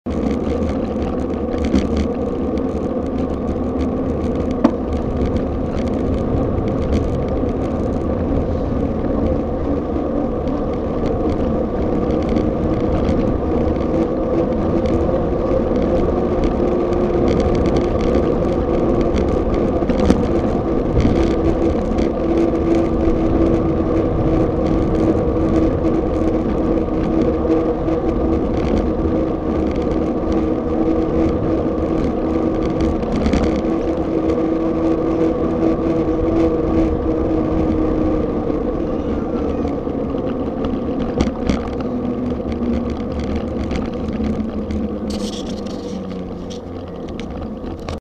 Contour+ test 1
Testing the handlebar mount on a little ride around Centennial park in Sydney. Nothing too eventful, although nearly got doored by a guy.. You know, the run of the mill standard stuff. Ride was 1/July/2013.

If the video is useful to anyone for any purpose - go nuts.